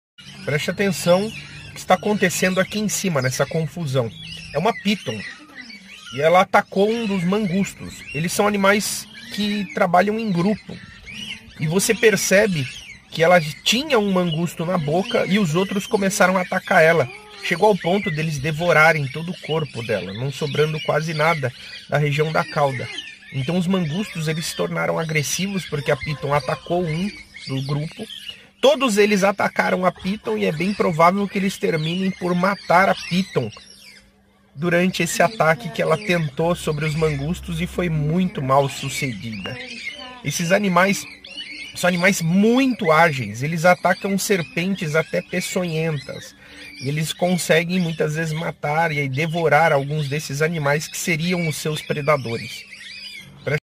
Mangustos devoram serpentes!
Ouça a breve explicação com o Prof. Dr. Atilio. Veterinário, Mestre e Doutor em Patologia. Professor de Medicina de Animais Selvagens, Patologia Animal e Fisiologia.

Este é um vídeo com intuito educacional. Caso se sinta prejudicado em algum sentido, envie uma mensagem privada.

This is an educational video. If you feel harmed in any way send me a private message.